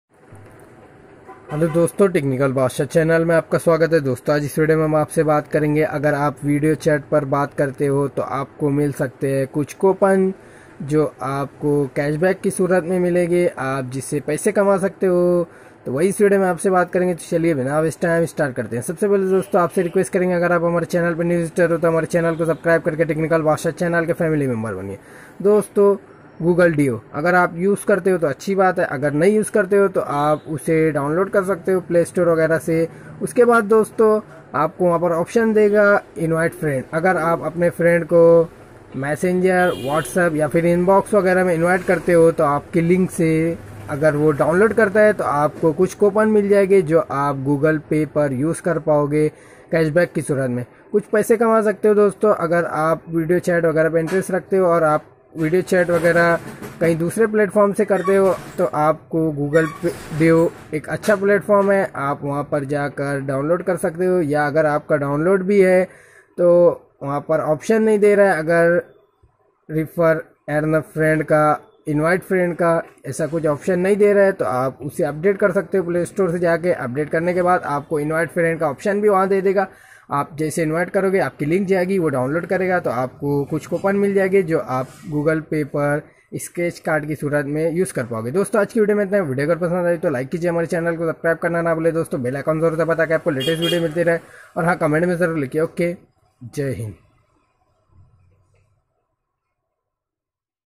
Video Call App Se Paisa Kamaoo
Hello Dosto
AJ is Video M Hum AAP Ko Btayenge video call app se paisa kamoo
Kise paisa Kamaoo get video m dekhoo jaldi se to video Ko last tak dekhte link niche di howi h
Pay app#

Jayada jankari ke liye video m dikhte Rhyee.
ok jai hind.